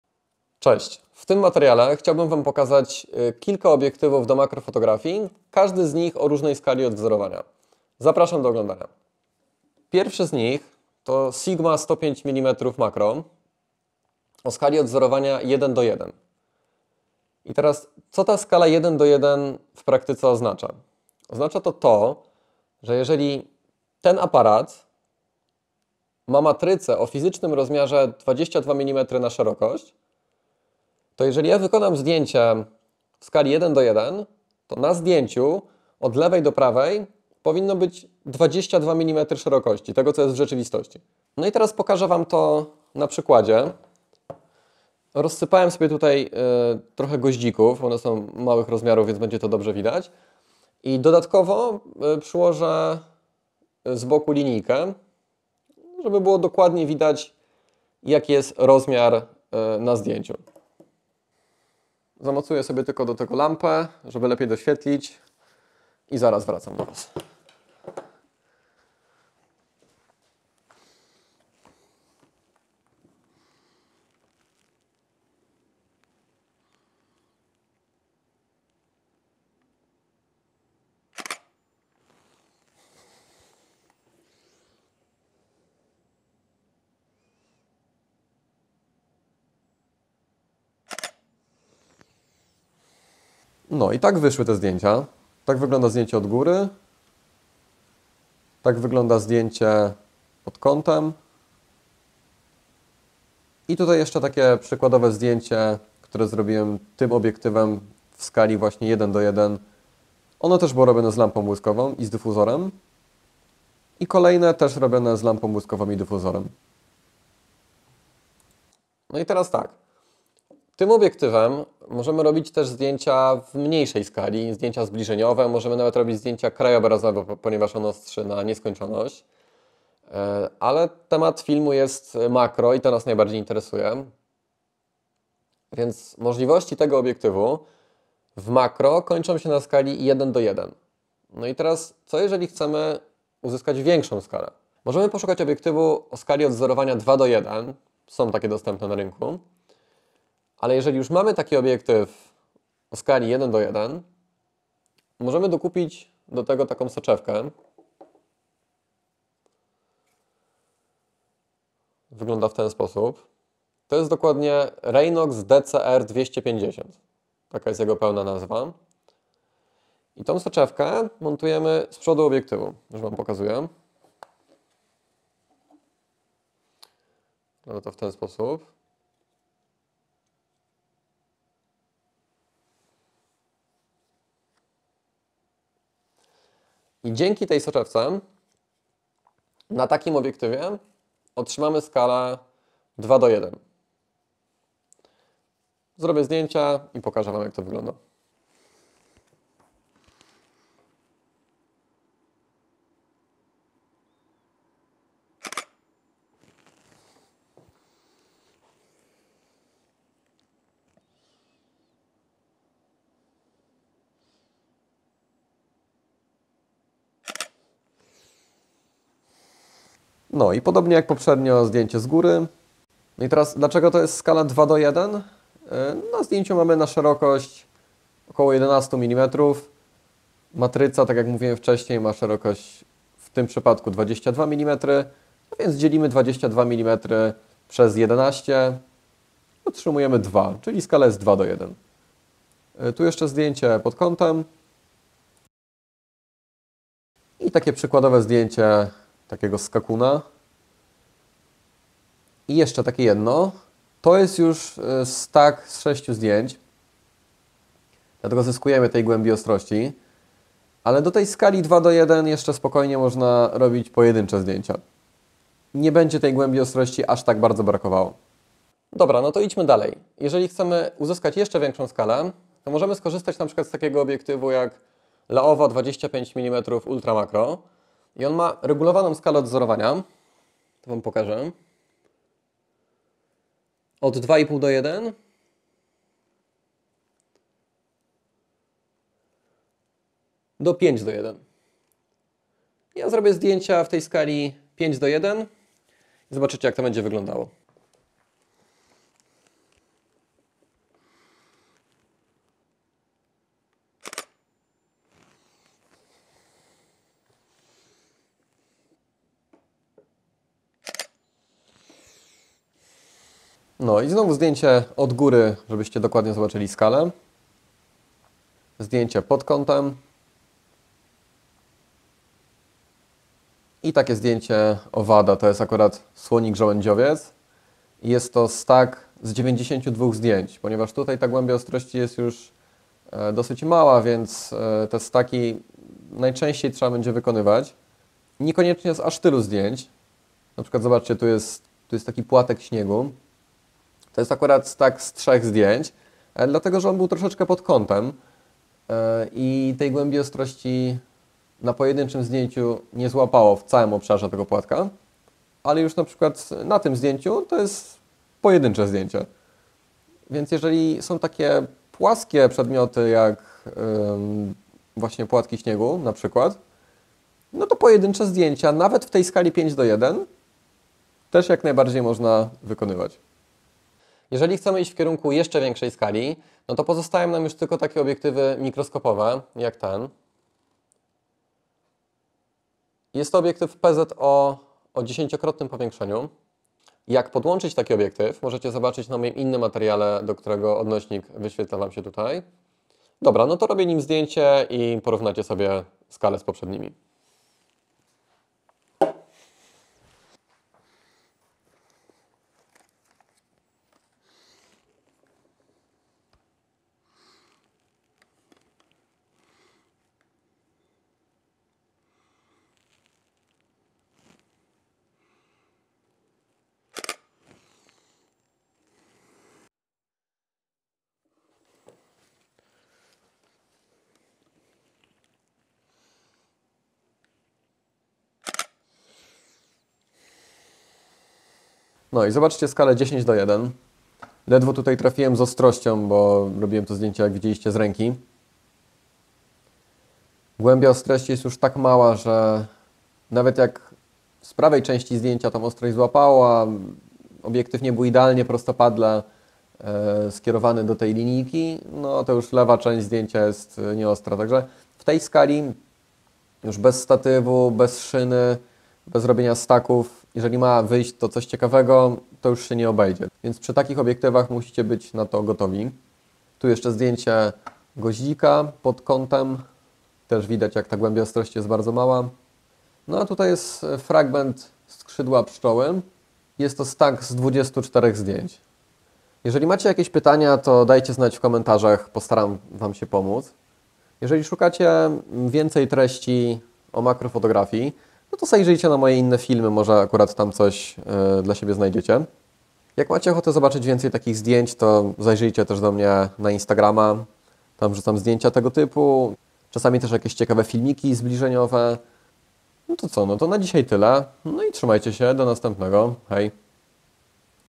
👽 Skala w makrofotografii. Przegląd obiektywów o różnej skali
Przegląd obiektywów o różnych skalach odwzorowania, pomiar skali, zdjęcia testowe i inne przykładowe zdjęcia.
Jeśli macie jakieś pytania, to dawajcie znać :)